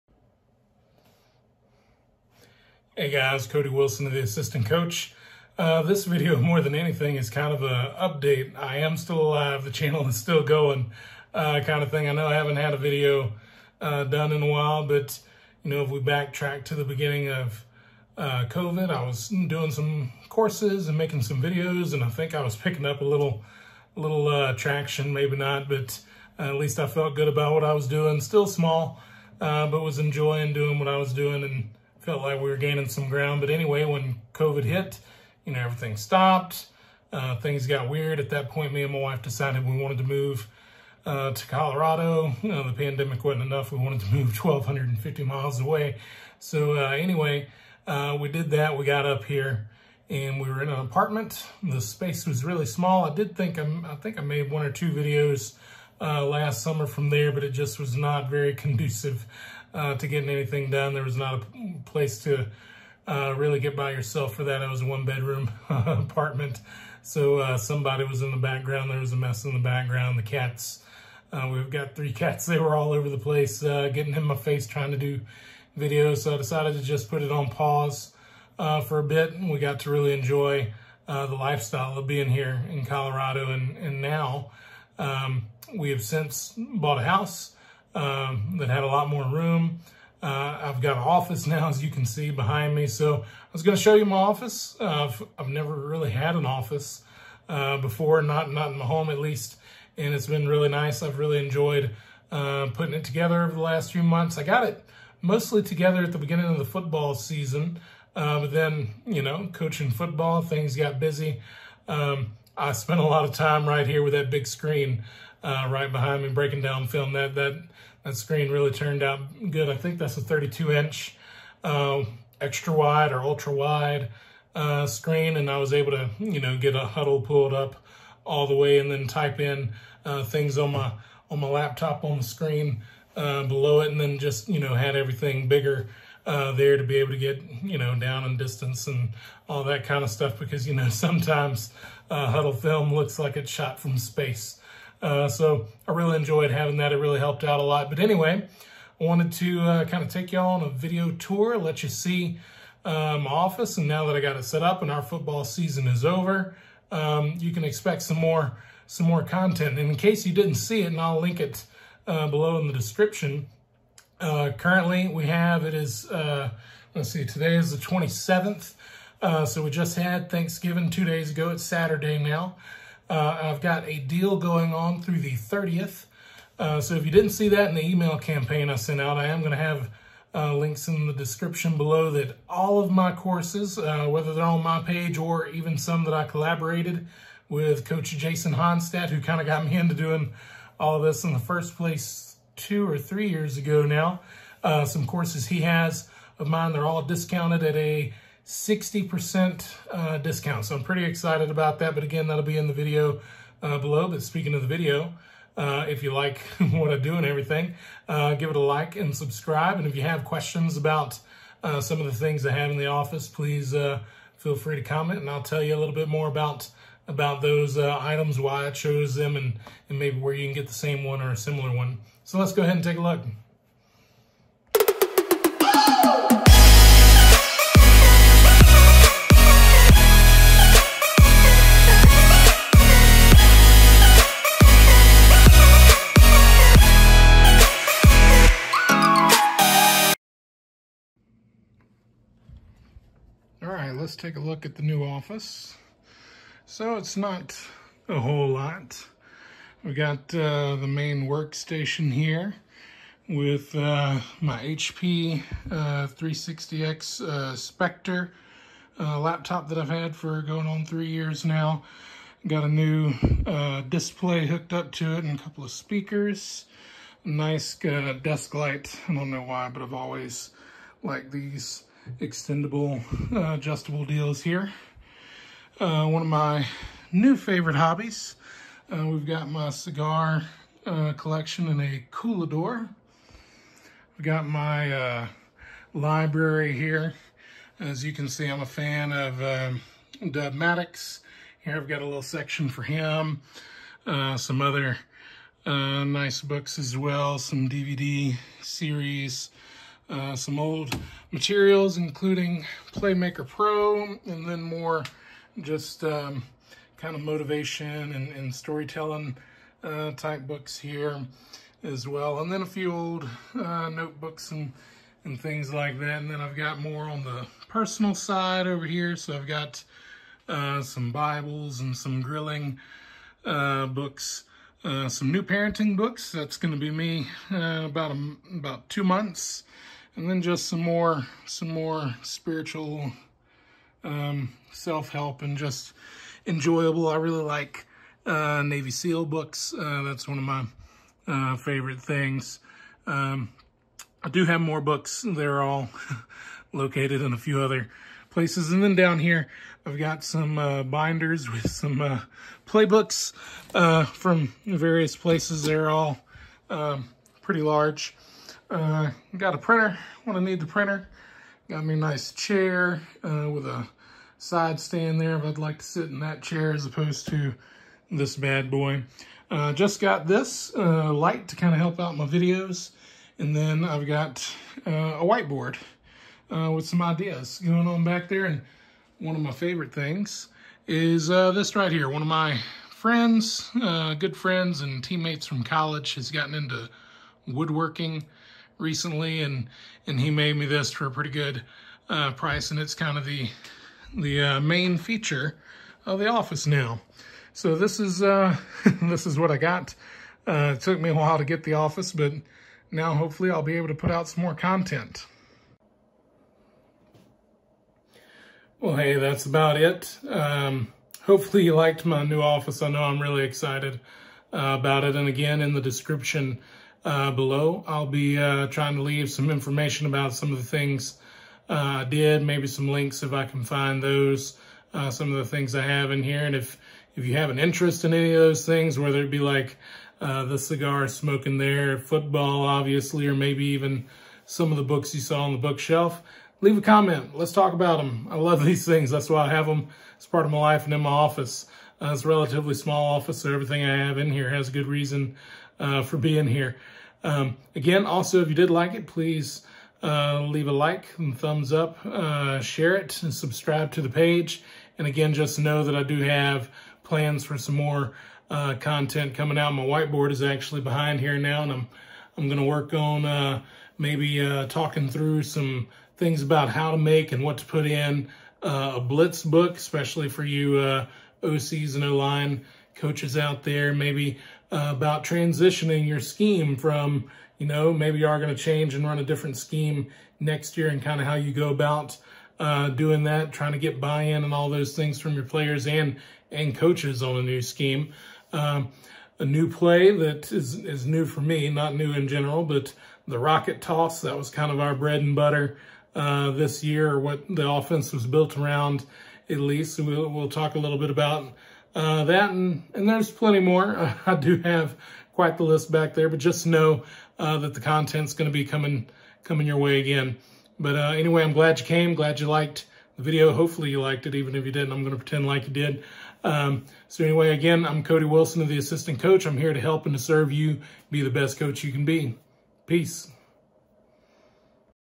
Tour My New Office
Now that I have a new office, more content should be coming out soon!

What it took to make my office.

Lowes Countertop  - $209.99
8-ft Honey Stained Straight Butcher Block Acacia Kitchen Countertop

Amazon Table Legs  - $79.97
CRIZTA Set of 2 Metal Table Legs 28 inch Height

Monitor   - $303.99
SAMSUNG 32 inch UJ59 4k monitor (LU32J590UQNXZA)

Monitor Mount  - $29.99
ErGear Monitor Mount for 15-32"

Desk Lamp  - $39.99
LEPOWER Metal Desk Lamp, Adjustable Goose Neck Architect Table Lamp

Desktop Speakers  - $19.99
Creative Pebble 2.0

Newair 250 Count Electric Cigar Humidor $400 (on sale)

Wood Flag Wall Mounted, 26 x 48 inches - $280
Weatherford Woodworking

Red Chair - $50
Facebook Marketplace